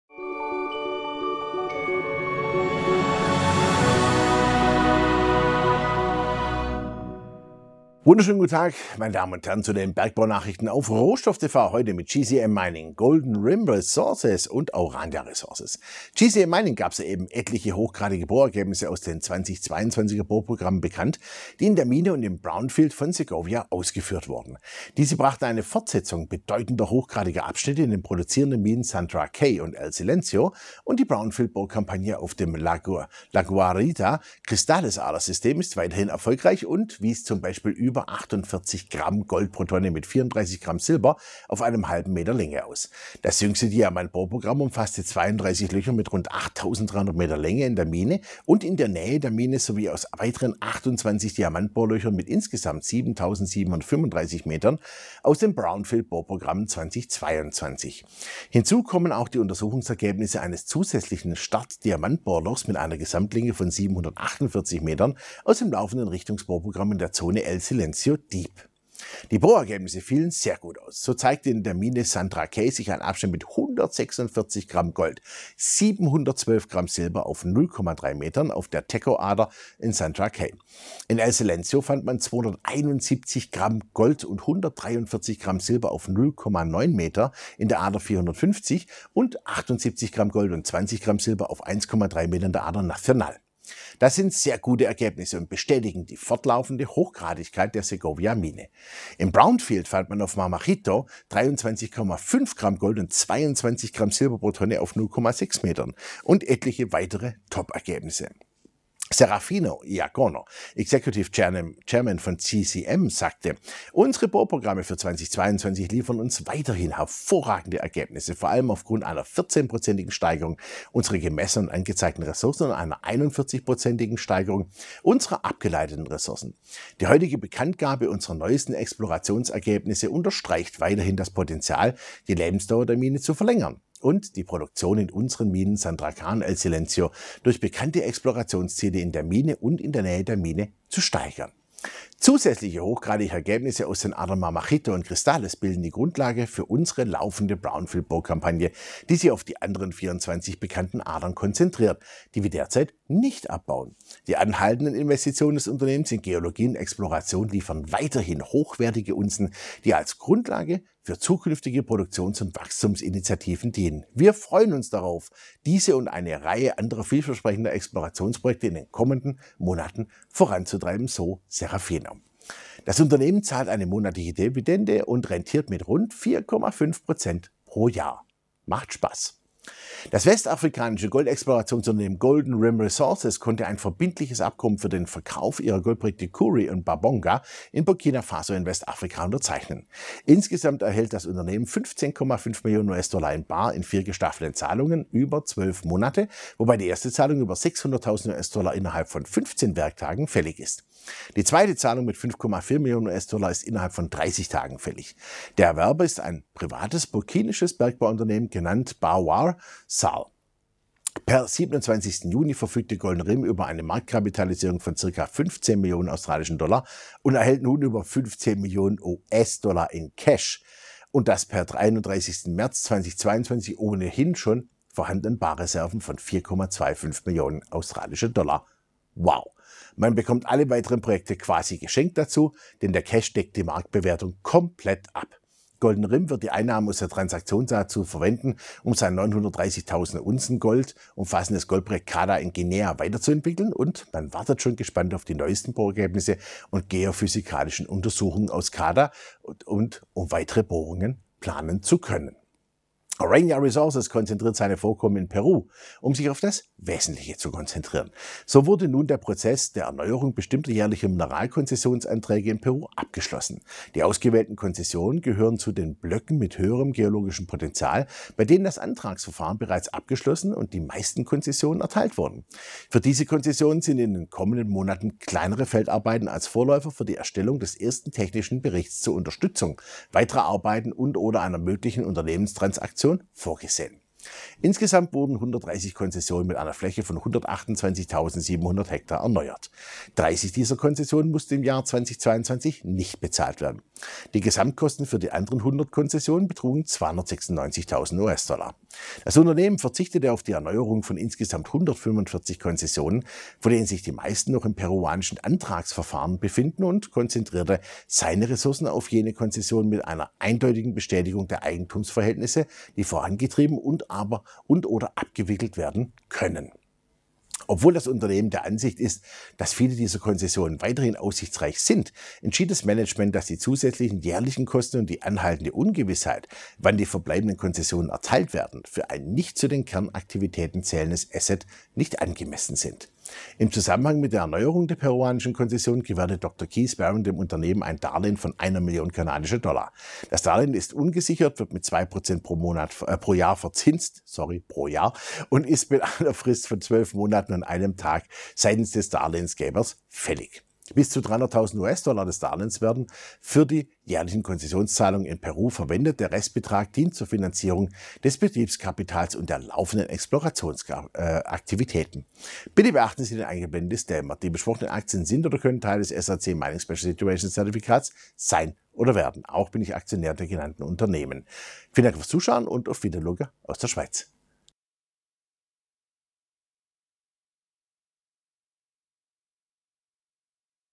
Bergbau-Nachrichten mit GCM Mining, Golden Rim Resources und Aurania Resources
GCM Mining hat soeben mehrere hochgradige Bohrergebnisse aus den 2022 durchgeführten Bohrprogrammen in der Mine Segovia und im Brownfield bekannt gegeben, das westafrikanische Goldexplorationsunternehmen Golden Rim Resources hat eine verbindliche Vereinbarung über den Verkauf seiner Goldprojekte Kouri und Babonga in Burkina Faso, Westafrika, unterzeichnet und Aurania Resources konzentriert sich auf seine Lagerstätten in Peru, um sich auf das Wesentliche zu konzentrieren. So ist das Verfahren zur Erneuerung bestimmter jährlicher Mineralkonzessionsanträge in Peru nun abgeschlossen.

0:00 GCM Mining
3:00 Golden Rim Resources
4:12 Aurania Resources

Want to ask direct questions to Mining stock CEO´s? Then participate in our FREE online investor talks.